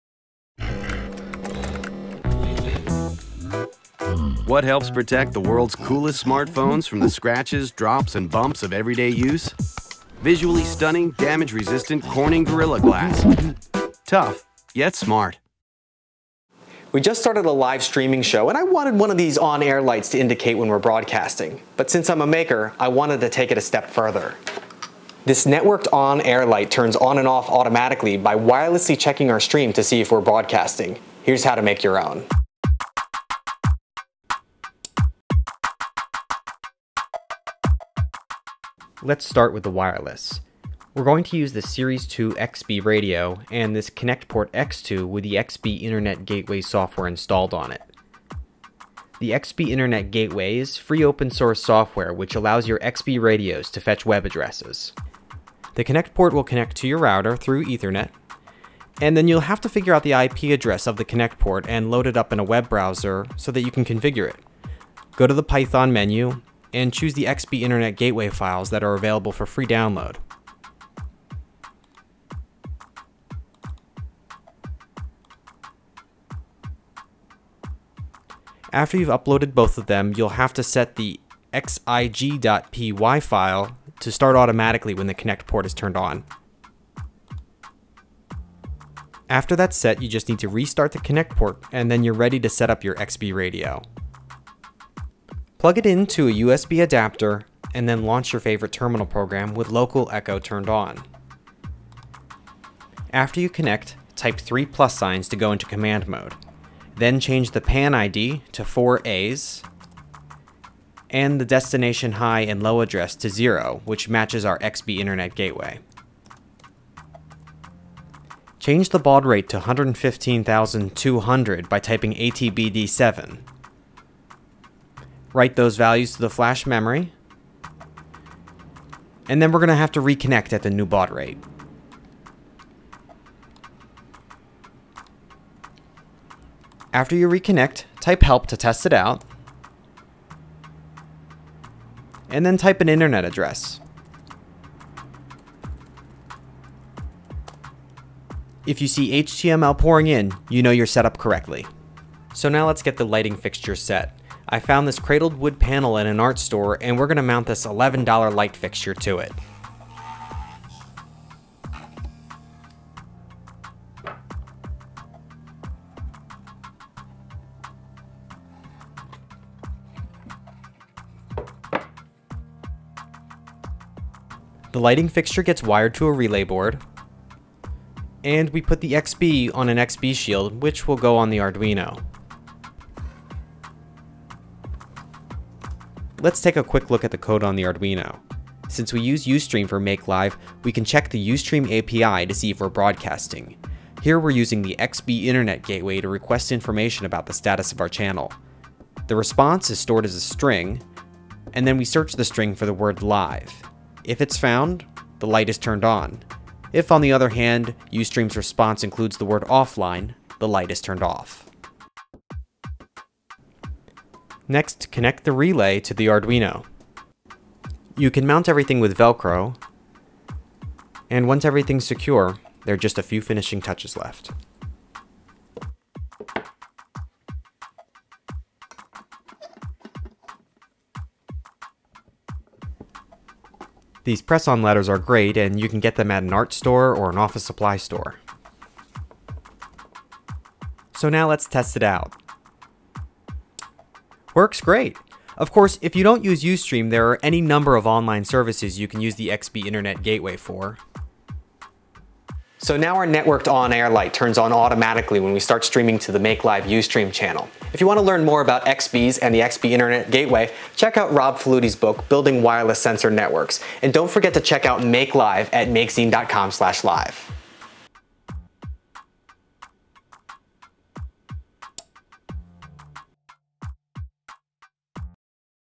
Networked On Air Light
You may have seen those "On Air" lights at film, radio or television studios. Now that anyone can broadcast video over the internet, it's time to learn how to build your own "Now Streaming" light that wirelessly checks to see if your streaming! You can use the basics of this project for any kind of wireless notifier.